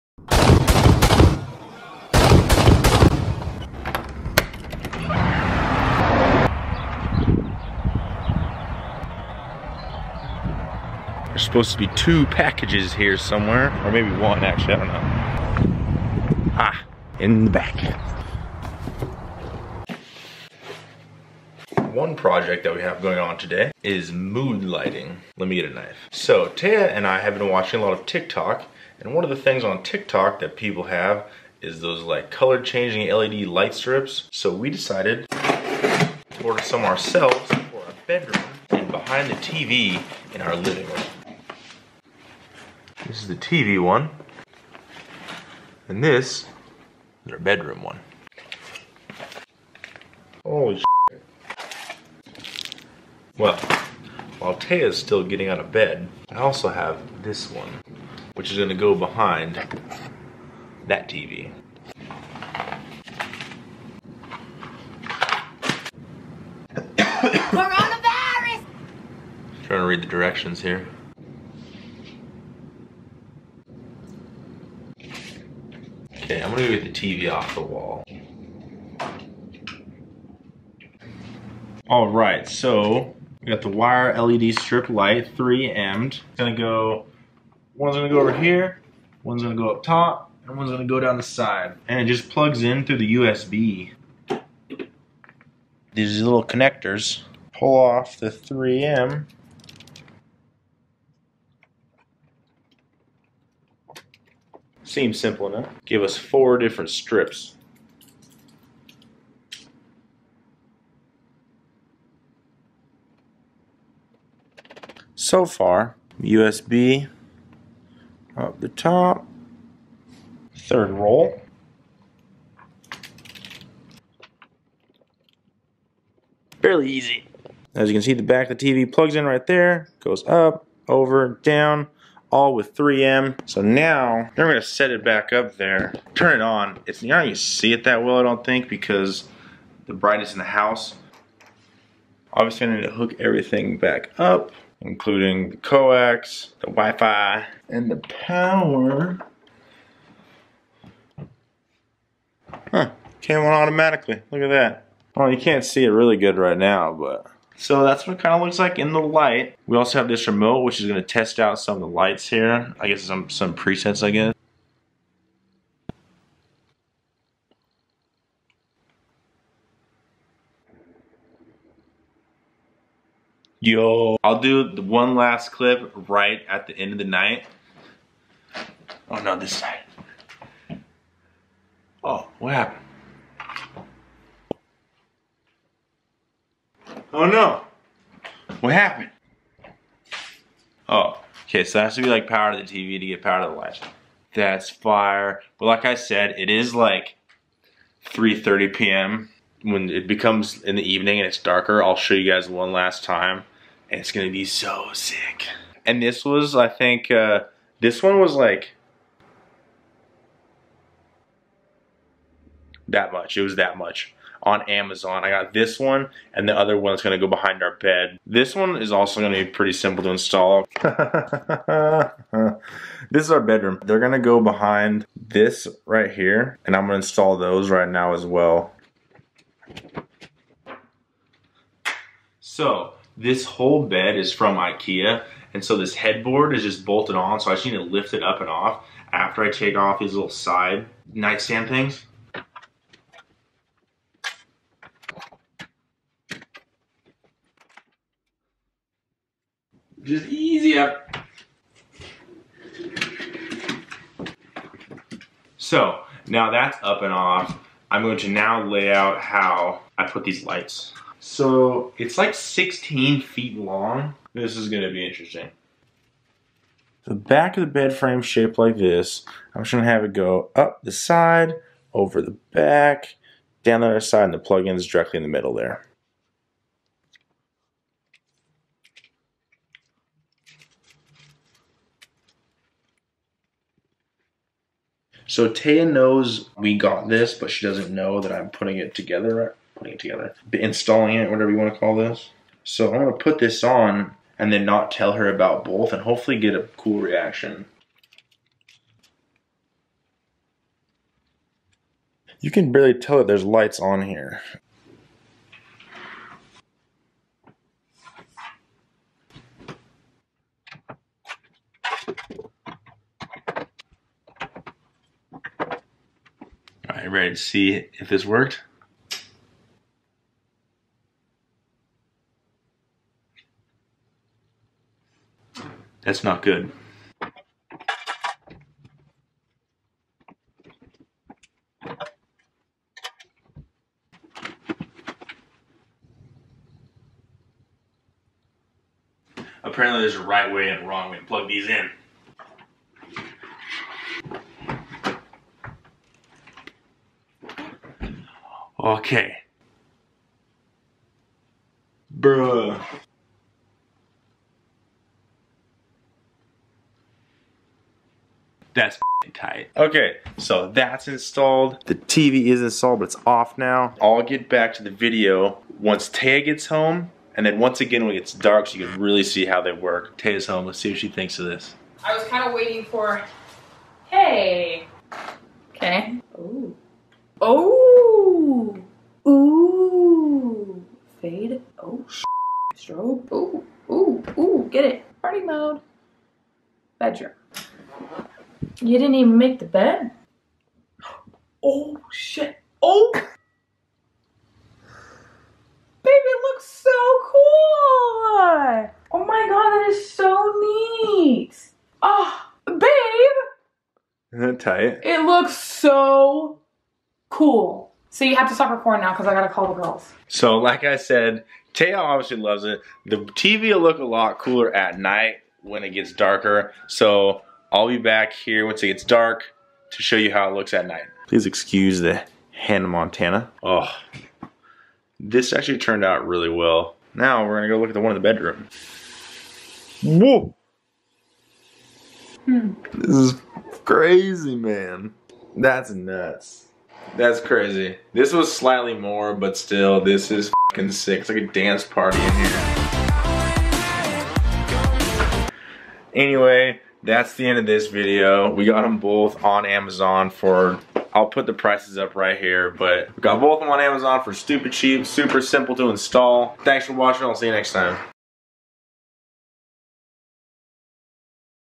Installing LED Lighting All Over My House
I bought some LED light strips and put them in my house

I have type 1 diabetes!
So check out my "Fuck Diabetes!" store as well also on lonelyvictor.com!

Send us Stuff:
Jesse Sellers
Greenacres, WA 99016